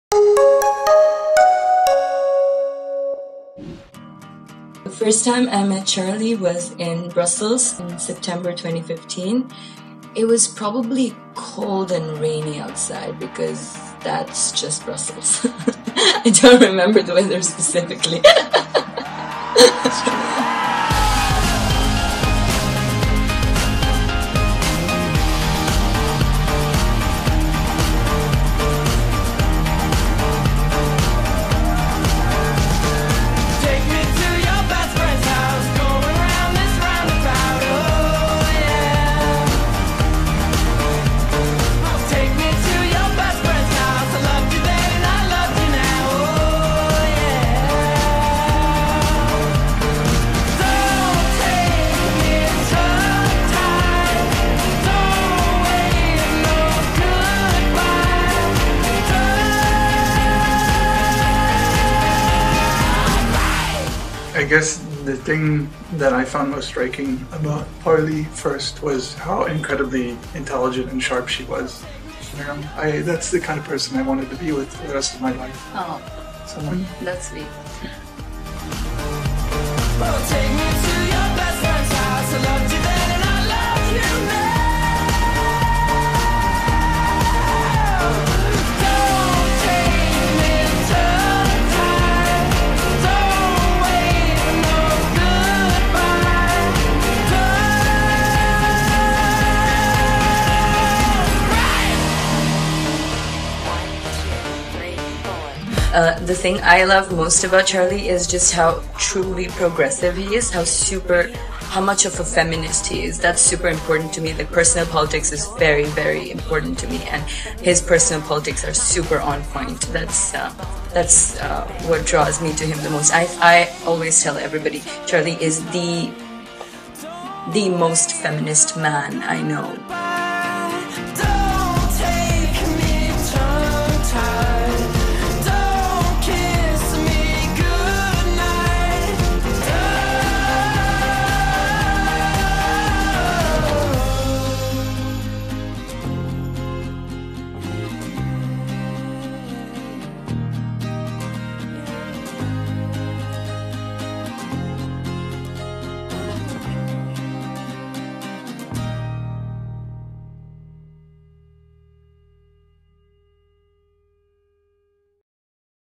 When a USA boy met an Assamese Girl || Shivangi weds Charles || Cinematic Wedding || Full HD
The beautiful journey of togetherness between Charles and Shivangi finally got blessed with the assamese wedding rituals in Guwahati. Charles, a govt. employee of The States and Shivangi, an assamese girl fall in love in Belgium, tied the knot in India and right now staying in USA. A great love story captured in frame.